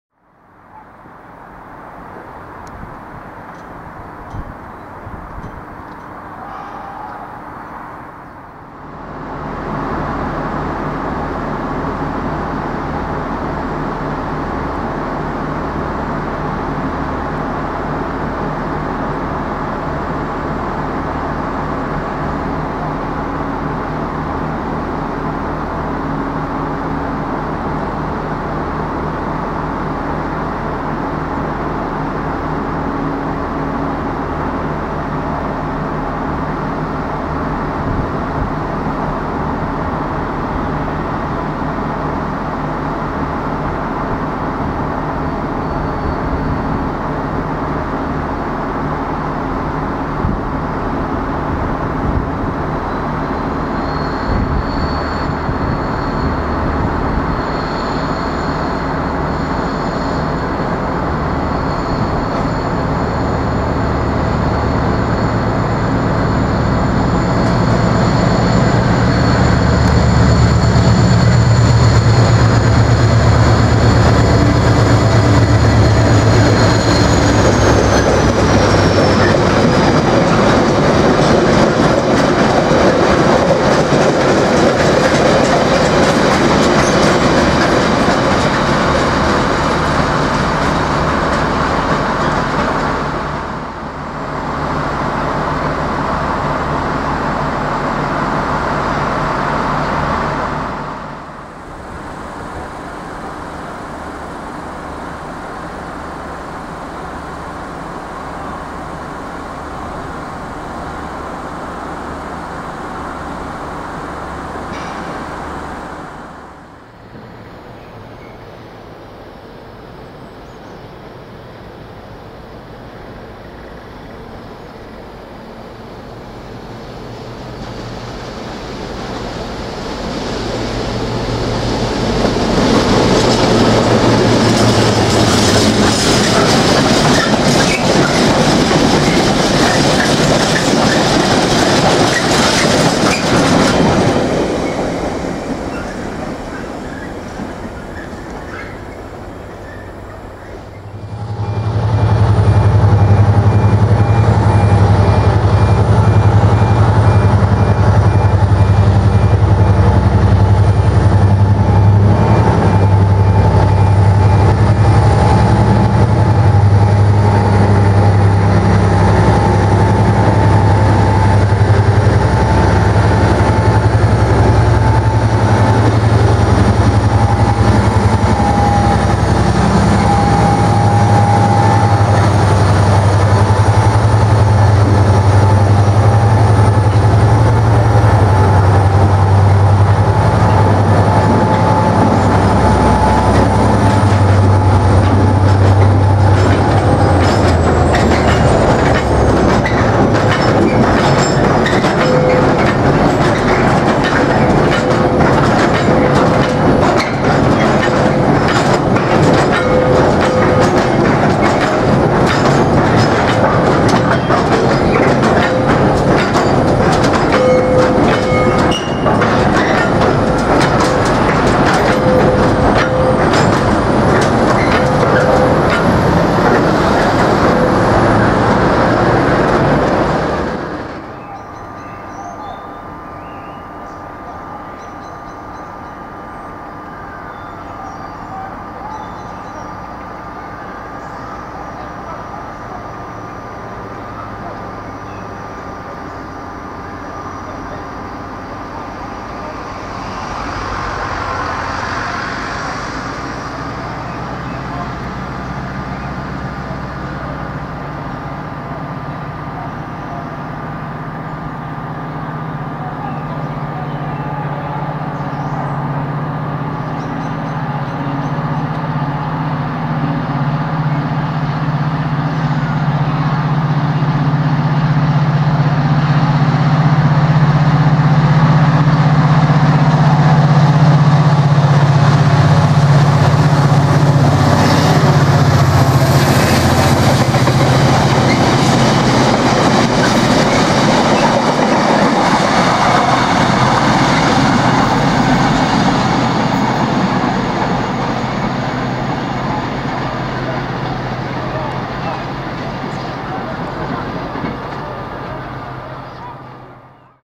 IE 077 & 074 on Tara Mines duties - 9th February 2015
Two of Irish Rail's class 071 locomotive were on Tara Mines duties for most of the month of February. The numbers of the loco's were 077 (With the new grey freight livery) and 074 (In the older Silver and Black freight livery).

The video starts off at Clontarf Road where we see 071 class locomotive number 077 passing with the 9:30 laden Tara Mines train.

Then after a nice cup of tea and a bite to eat we head up to Ashbrook where we see 077 again with the 14:00 empty Tara Mines train to Tara Mines Co. Meath.

Then we see 071 class locomotive number 074 slowly making its way down Alexandra Road with the 12:30 laden Tara Mines train from Tara Mines, Co. Meath.

Later on we head back to Clontarf Road where we see 074 again with the late running 17:45 empty Tara Mines train, this train was running 35 minutes late.

All footage was taken on Monday the 9th of February 2015.